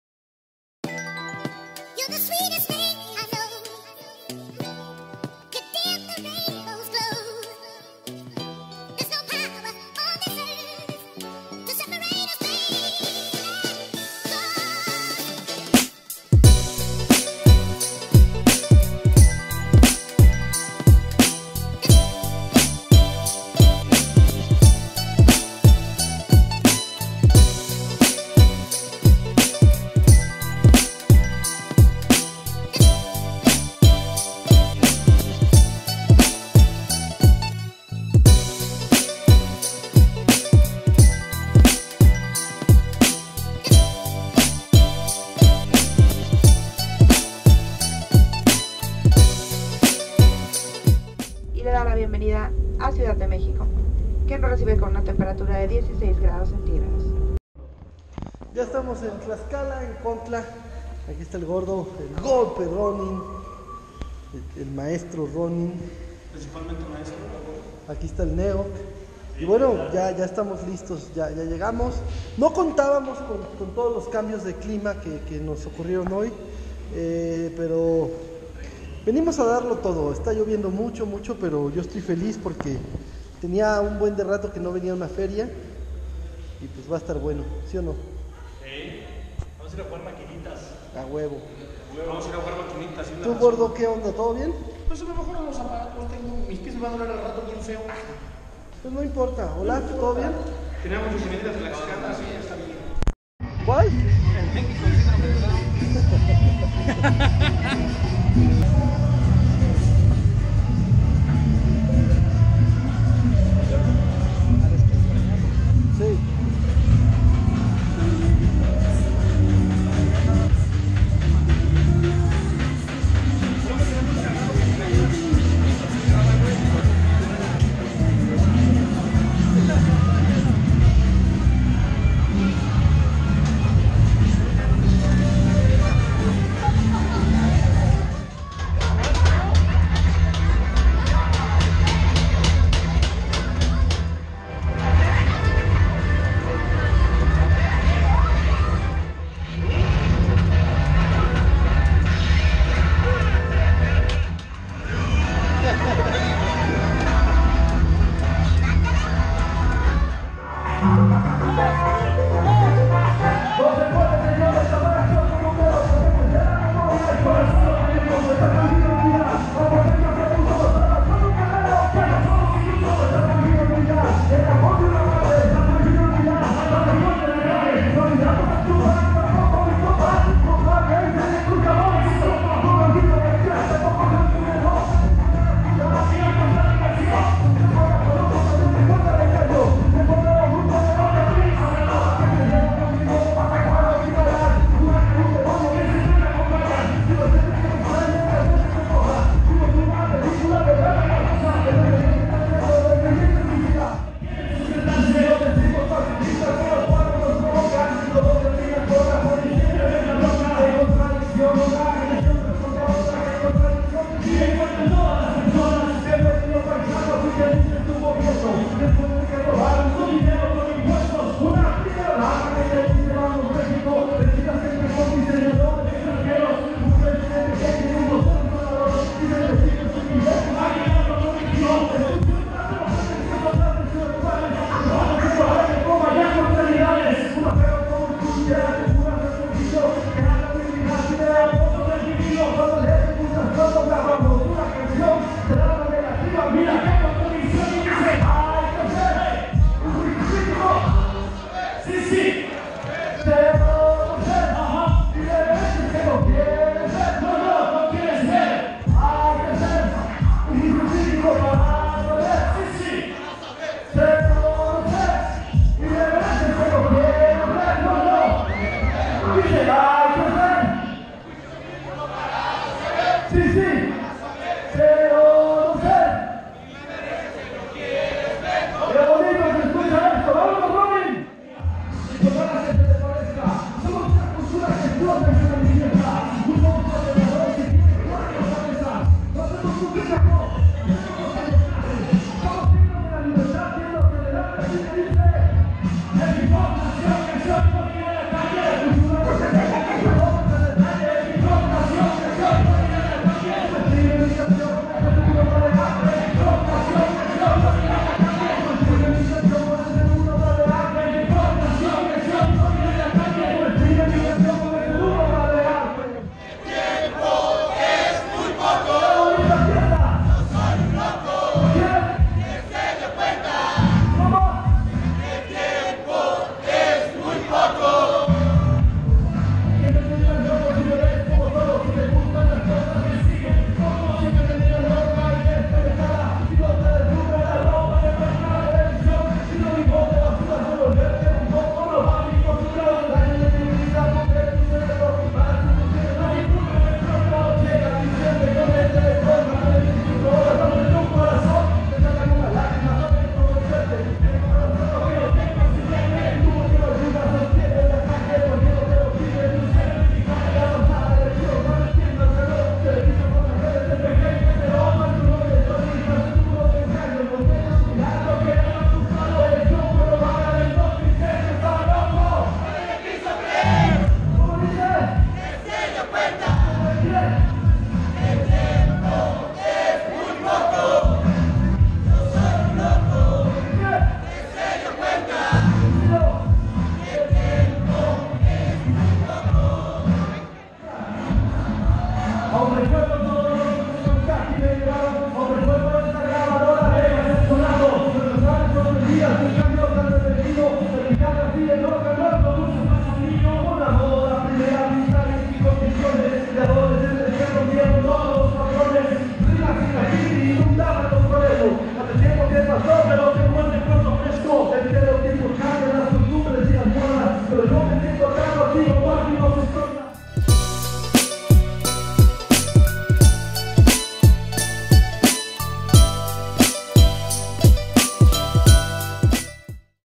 El podcast de Skool 77 Capitulo 1
En este episodio de 7 minutos les presentamos imágenes de nuestra visita por Contla Tlaxcala. Acompañado de Neock y Golpe el Ronin en los platos.